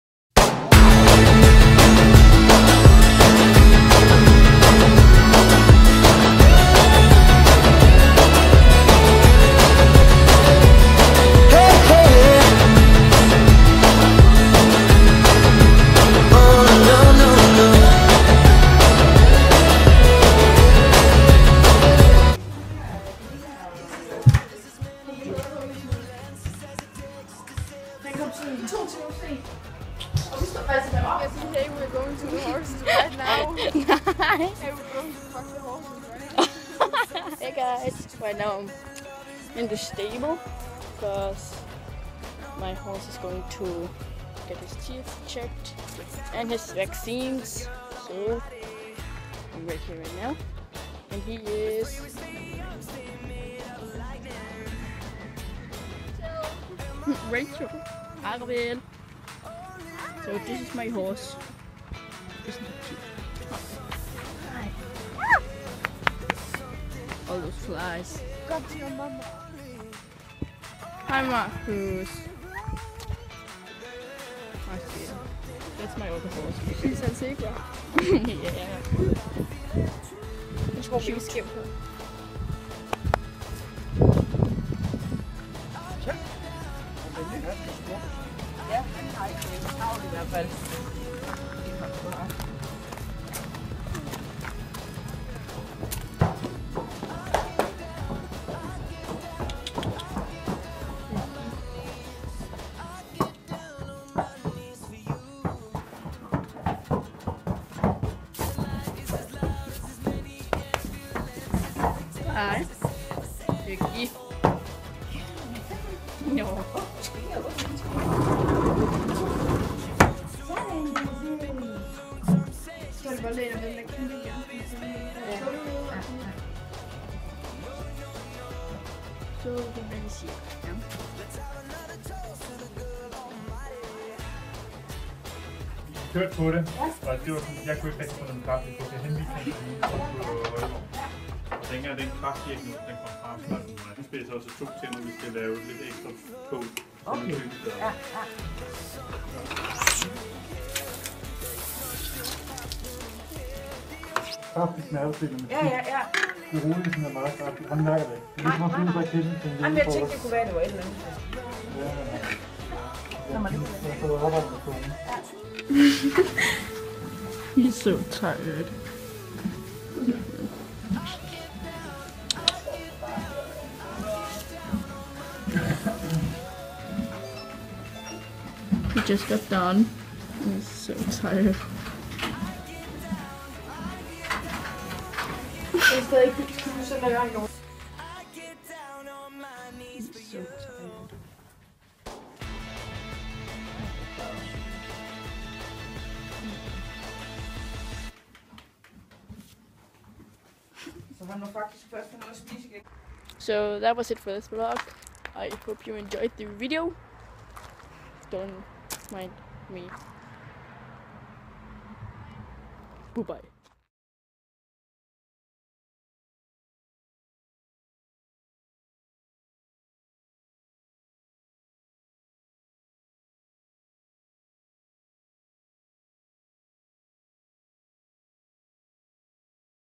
Barn Vlog - Ravel and the vet??
So today Ravel got his teeth checked and stuff, hope you enjoyed the video
Deviantart - Thincici12
Skype - zeep-star
Have a great day! :3